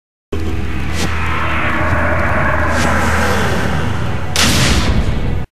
Acosh hellsing
It's me drawing a model gun in hellsing style.